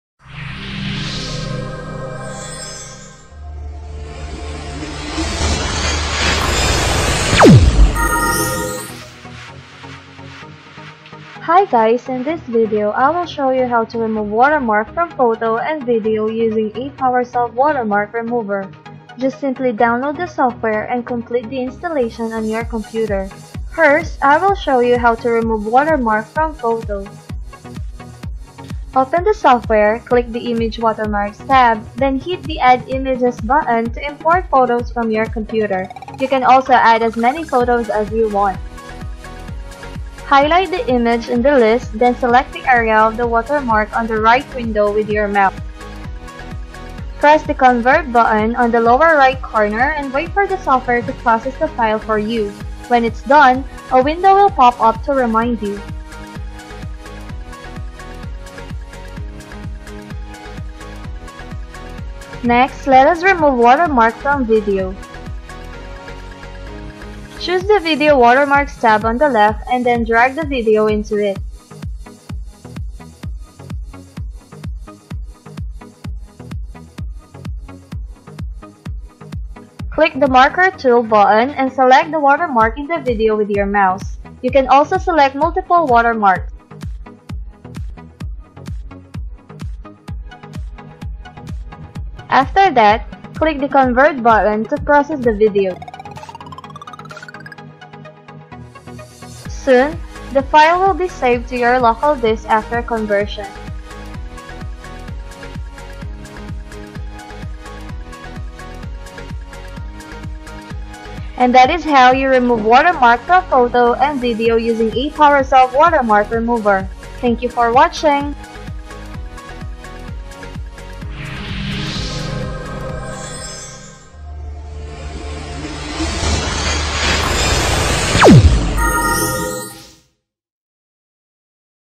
How to use Apowersoft Watermark Remover
Watch this video tutorial of Apowersoft Watermark Remover and you'll know how to remove watermark from video and photo quickly and easily.